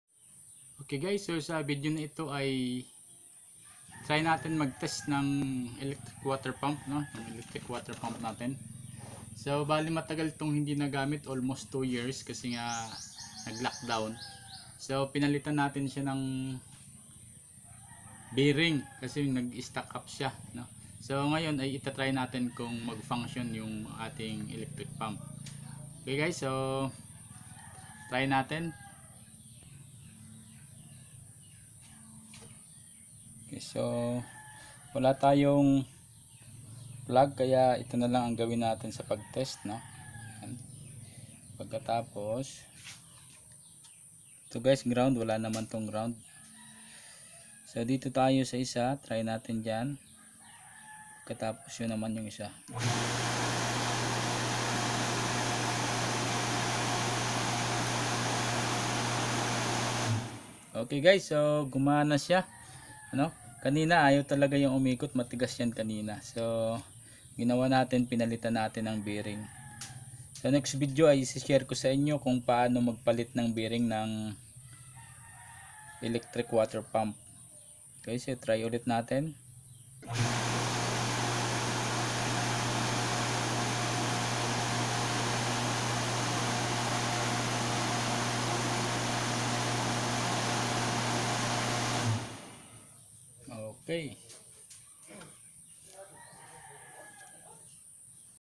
HOW TO TEST ELECTRIC WATER PUMP
In this video i will show you how to test electric water pump..